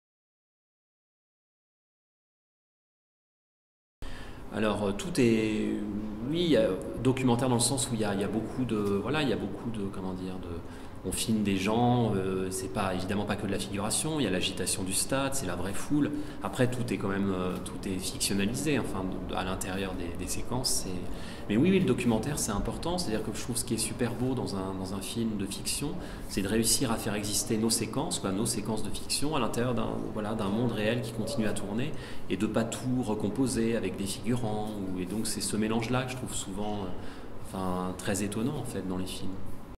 Prix Jean Renoir des lycéens 2019 : l'aspect documentaire dans le film de Mickaël Hers
Pourquoi Mickaël Hers donne-t-il un aspect documentaire à son film "Amanda" ?